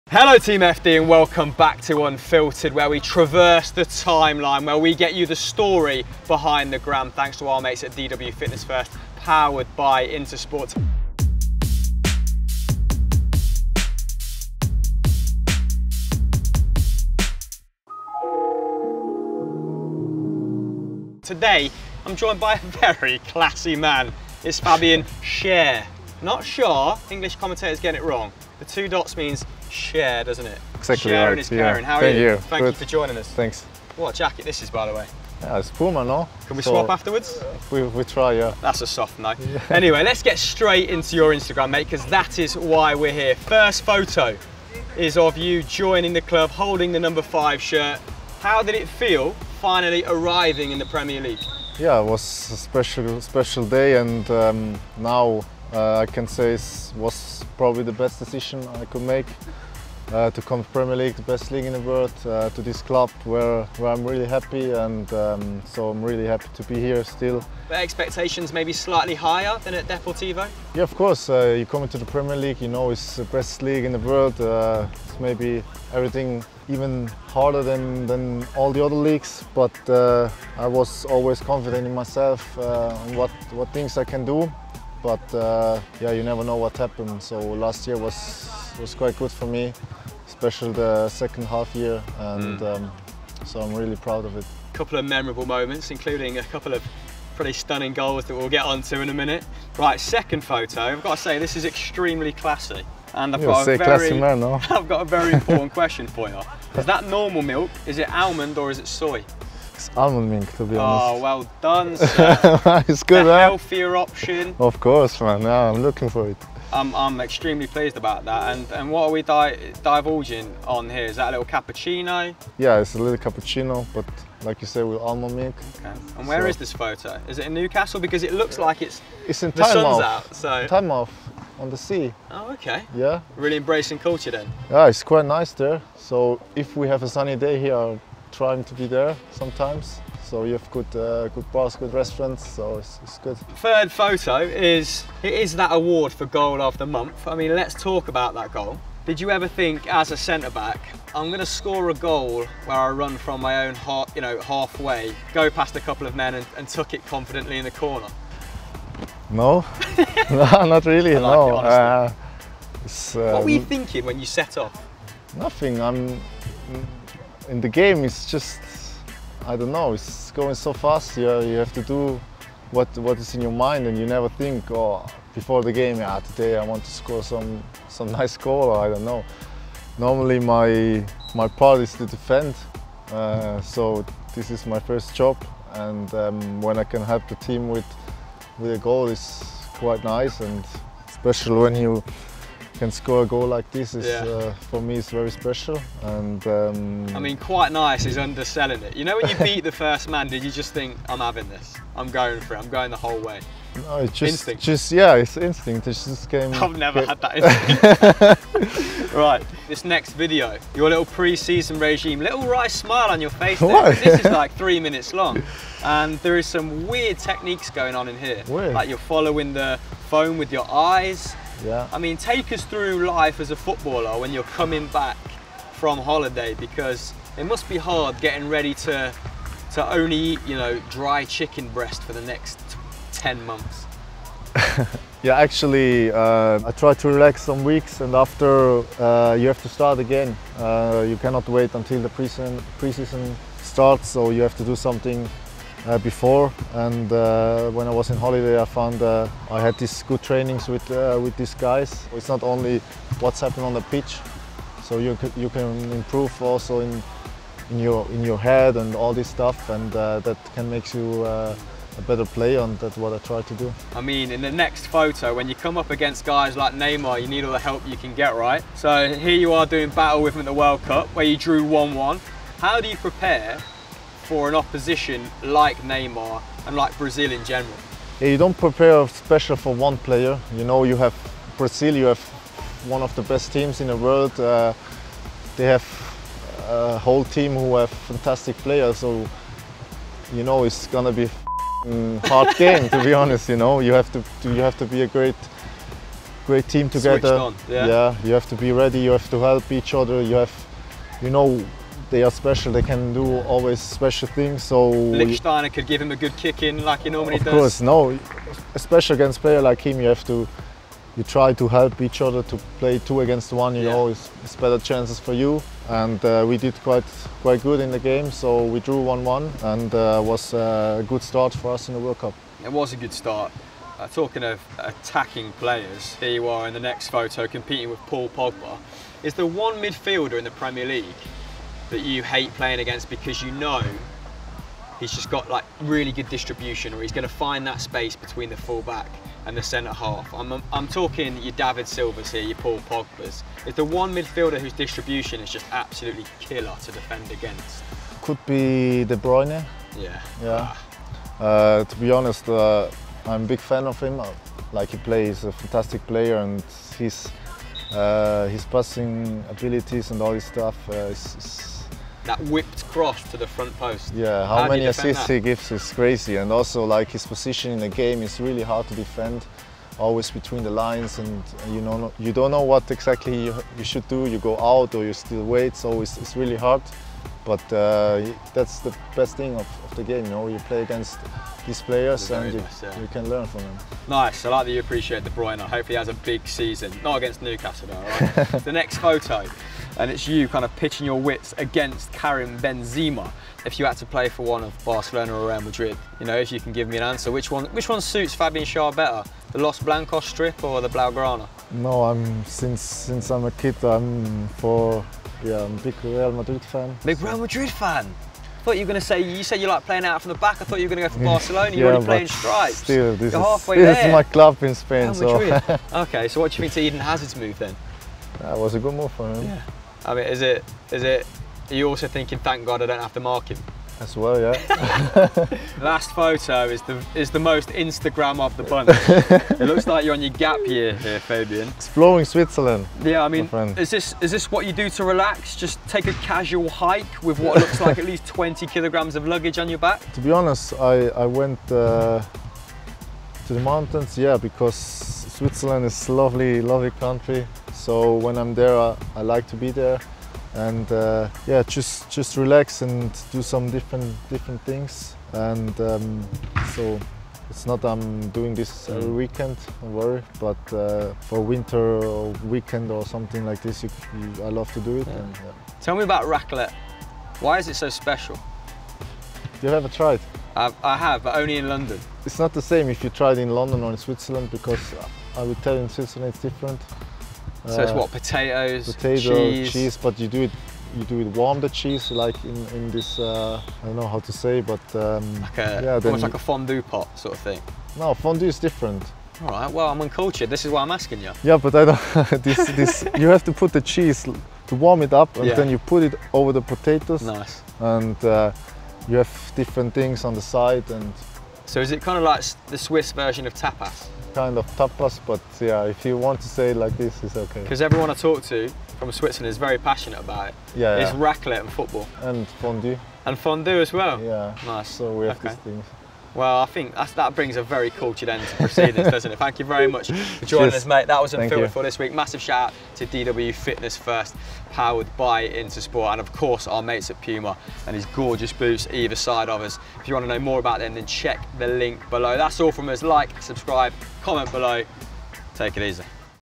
“KEVIN DE BRUYNE IS THE WORST PLAYER TO PLAY AGAINST!” Ft. Fabian Schar
Newcastle & Switzerland defender Fabian Schar talks us through his Instagram account.